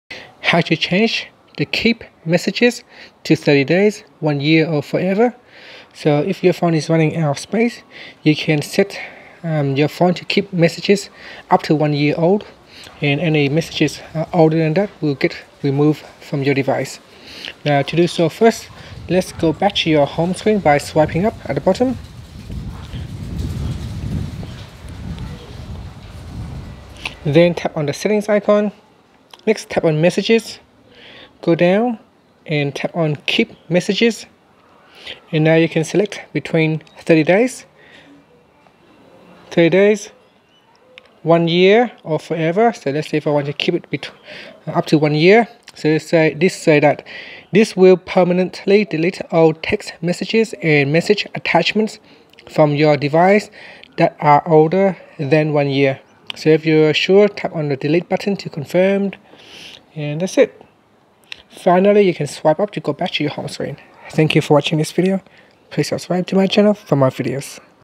iPhone 11 Pro: How to Change Keep Messages for 30 Days / 1 Year / Forever | iOS 13
Learn how you can change to keep your messages for 30 days, 1 years or forever on iPhone 11 Pro.

iOS 13.